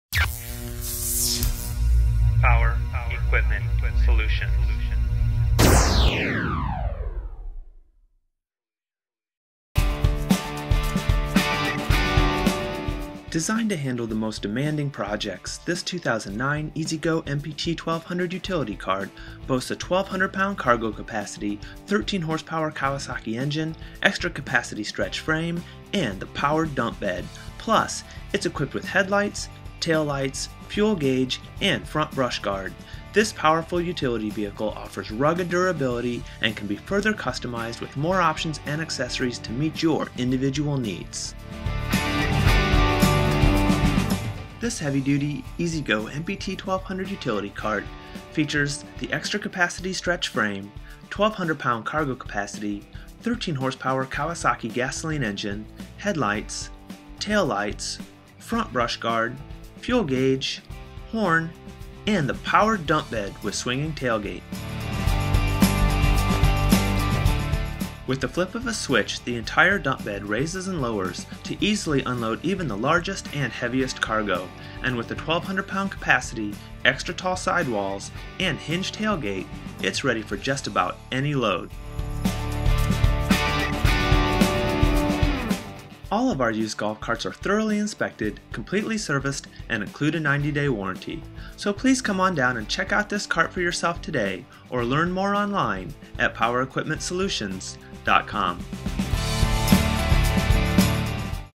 2009 E-Z-GO MPT 1200 Utility Cart
Designed to handle the most demanding projects, this 2009 E-Z-GO MPT 1200 utility cart boasts a 1200 lb. cargo capacity, 13 HP Kawasaki engine, extra-capacity stretch frame, and the powered dump bed.  Plus it is equipped with headlights, tail lights, fuel gauge, horn, and front brush guard.  This powerful utility vehicle offers rugged durability and can be further customized with options and accessories to meet your individual needs.